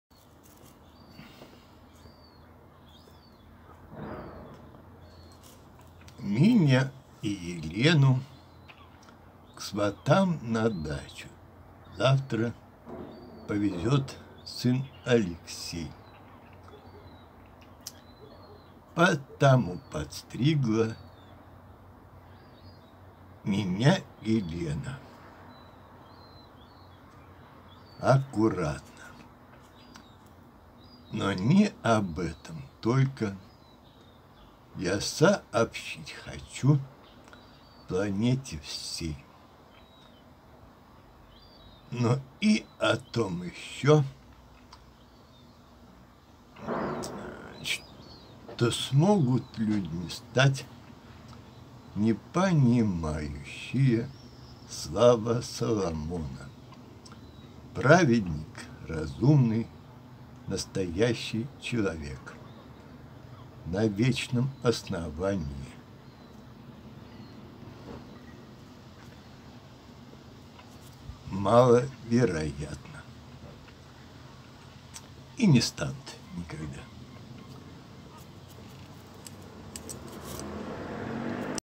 Что люди людьми станут, маловероятно
Меня и Елену к сватам на дачу завтра повезет сын Алексей.
Потому подстригла меня Елена аккуратно.
Но не об этом только я сообщить хочу планете всей.
Но и о том еще, что смогут людьми  стать непонимающие слова Соломона — «...праведник (разумный, настоящий человек) — на вечном основании» — маловероятно.